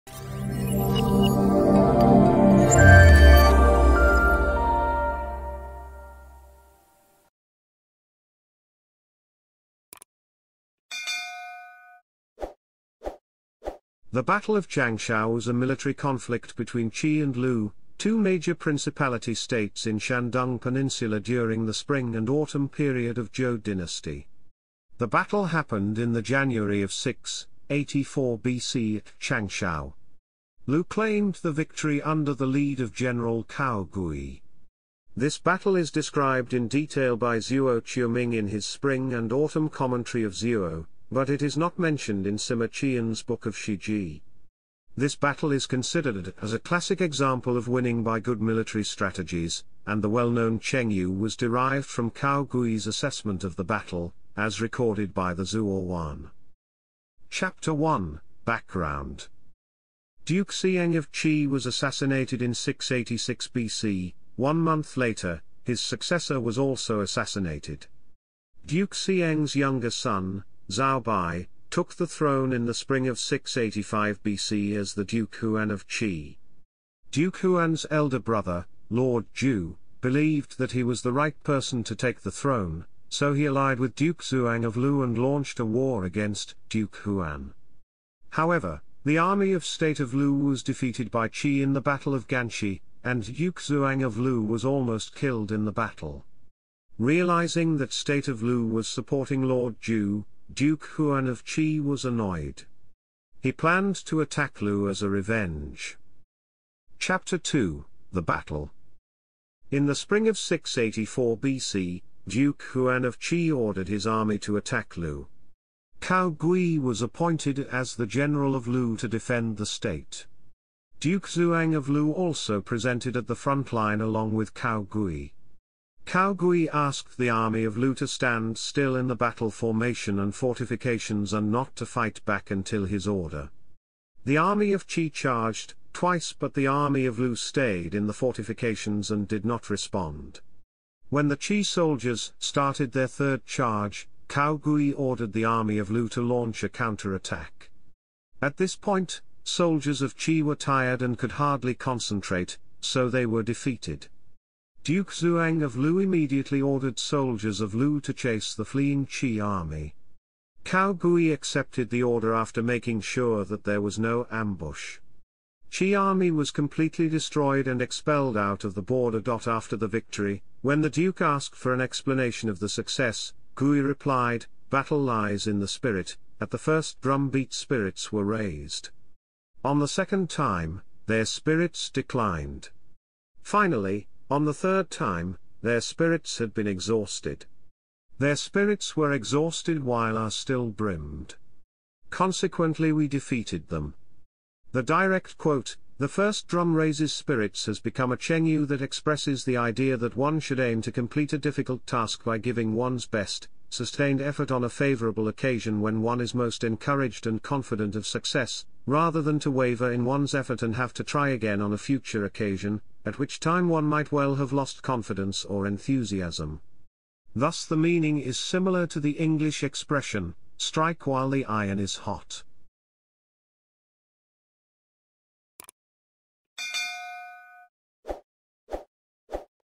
Battle of Changshao - Wikipedia article
Video summary:
The Battle of Changshao was a military conflict between Qi and Lu, two major principality states in Shandong Peninsula during the Spring and Autumn Period of Zhou dynasty. The battle happened in the January (lunar calendar) of 684 BC at Changshao. Lu claimed the victory under the lead of general Cao Gui (曹劌). This battle is described in detail by Zuo Qiuming in his Spring and Autumn Commentary of Zuo (Zuozhuan), but it is not mentioned in Sima Qian's book of Shi Ji. This battle is considered as a classic example of winning by good military strategies, and the well-known chengyu 一鼓作氣 (yī gŭ zuò qì, lit: "first drum creates spirit", explained below) was derived from Cao Gui's assessment of the battle, as recorded by the Zuozhuan.